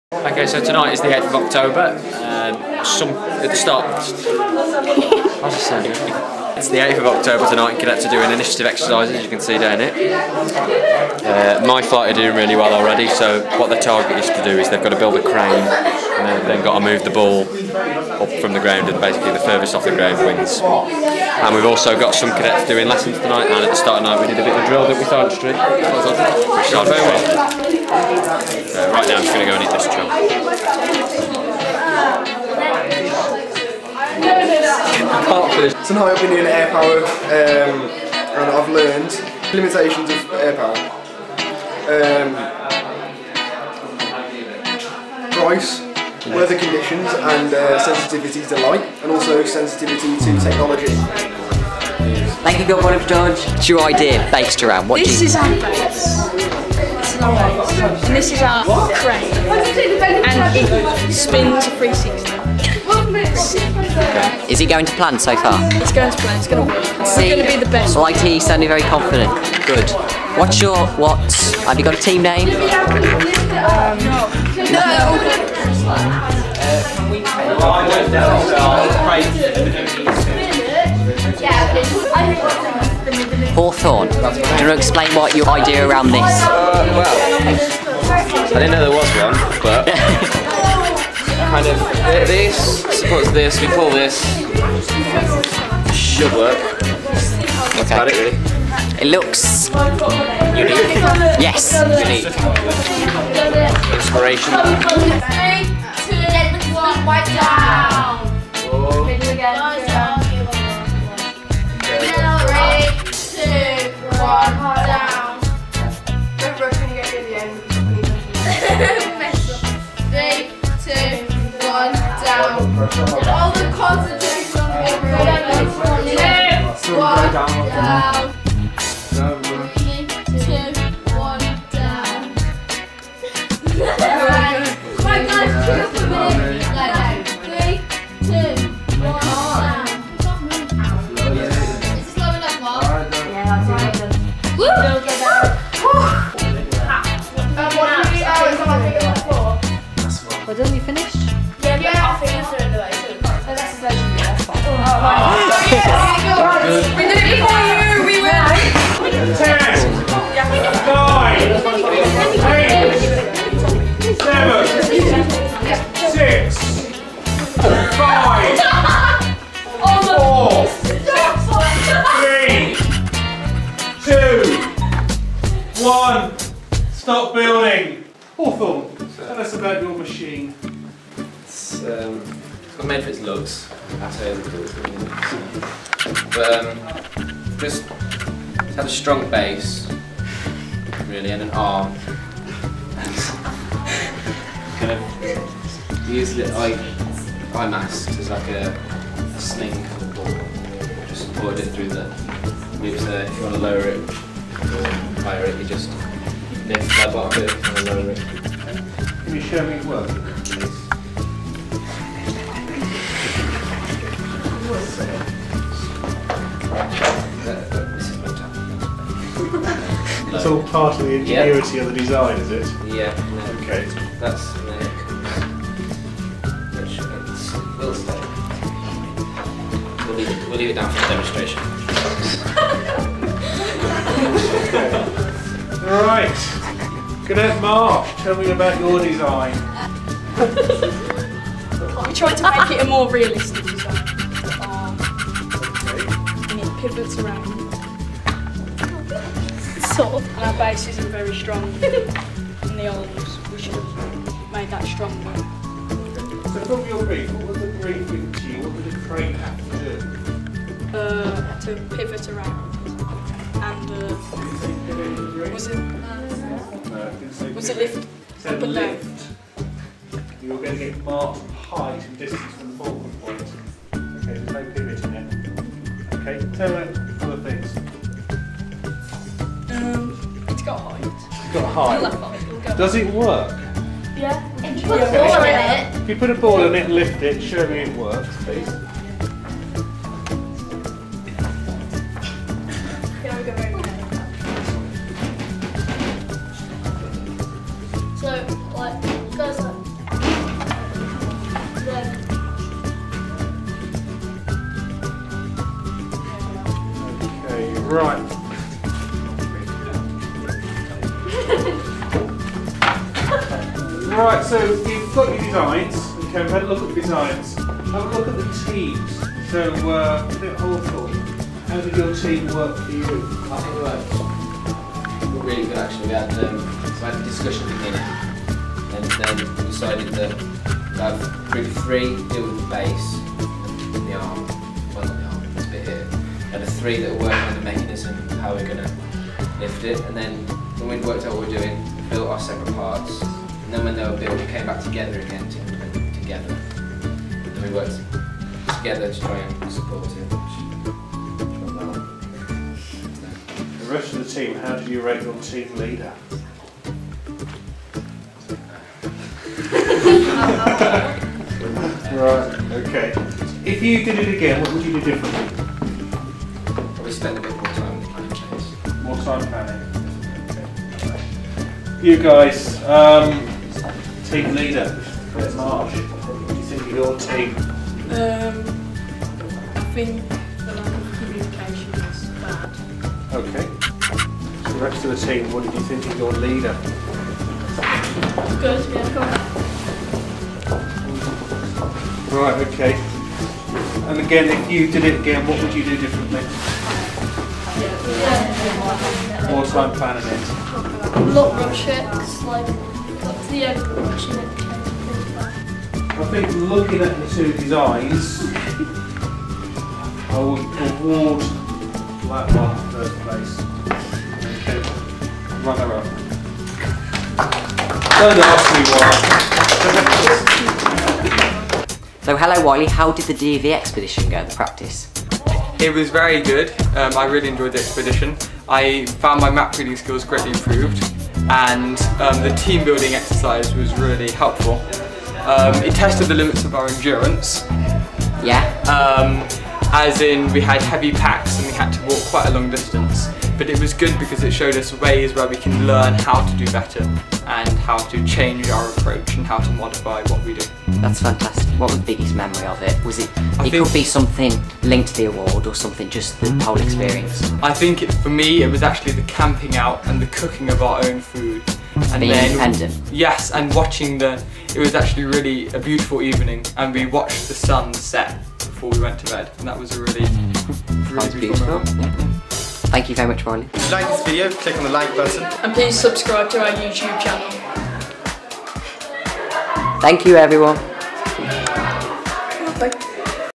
Vlog #11 - What a challenge
Once again, the cadets are competing to win two challenges set by staff and others have been learning about Air Power.

Camera: Sergeant Ryan Phillips
Edited by: Sergeant Ryan Phillips